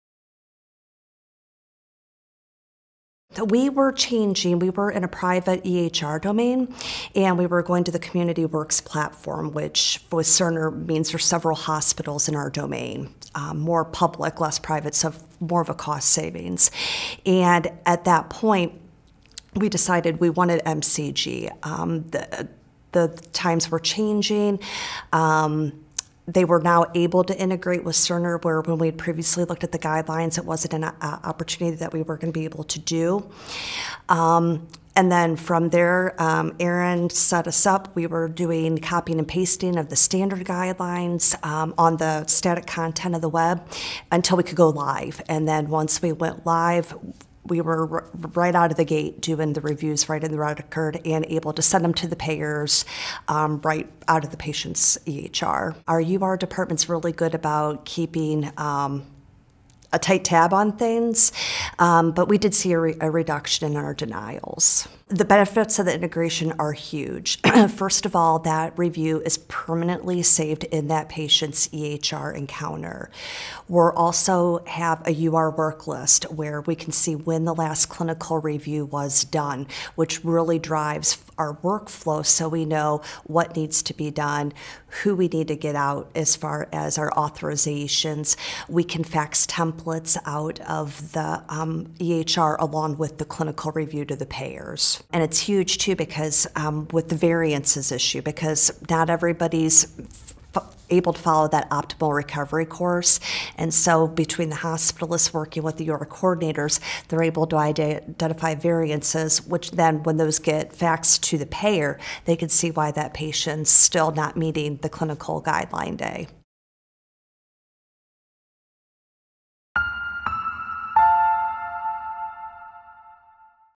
MCG Integrated with Cerner
Melanie Hoover, MSN, RN, CNOR, the Quality Director for Magruder Hospital in Ohio, discusses how her hospital revolutionized their utilization review workflow by integrating MCG evidence-based content into their Cerner EHR. Learn more about how MCG can help healthcare providers